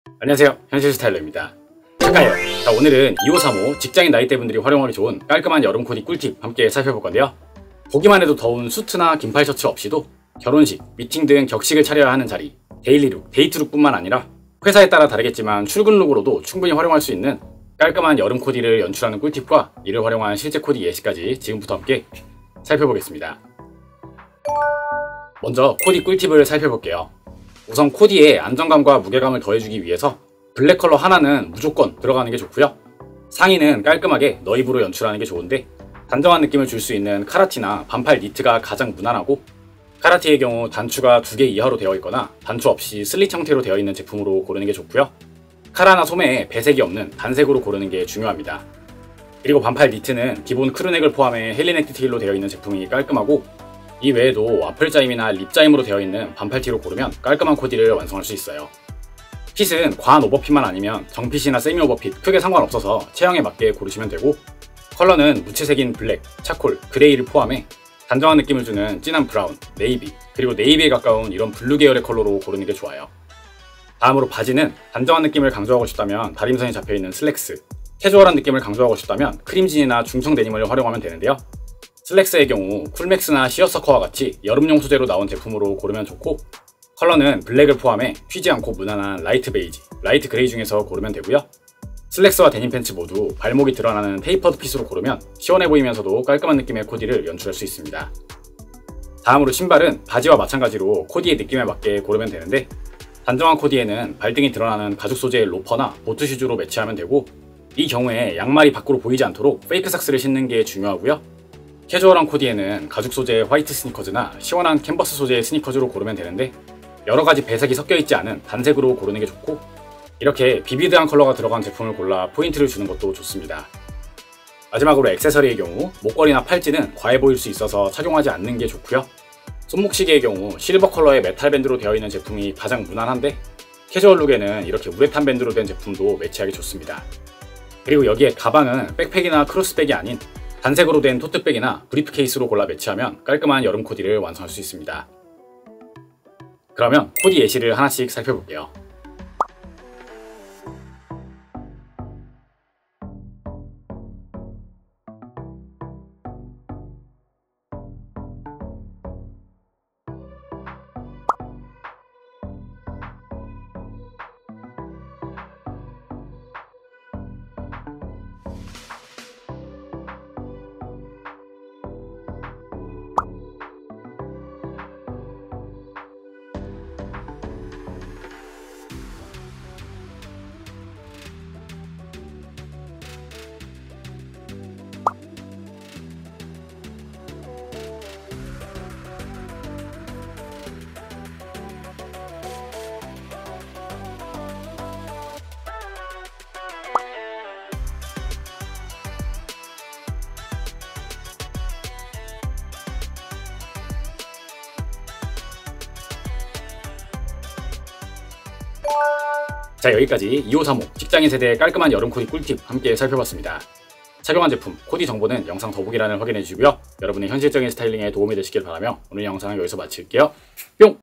직장인 20대 30대 깔끔한 여름코디 꿀팁! (소개팅, 결혼식, 캐주얼)
보기만해도 더운 수트, 셔츠 없이도
결혼식, 미팅 등 격식을 차려야하는 자리,
데일리룩, 데이트룩 뿐만 아니라
출근룩으로도 충분히 활용할 수 있는
깔끔한 여름 코디를 연출하는 꿀팁과
실제 코디 예시까지 함께 살펴보겠습니다😊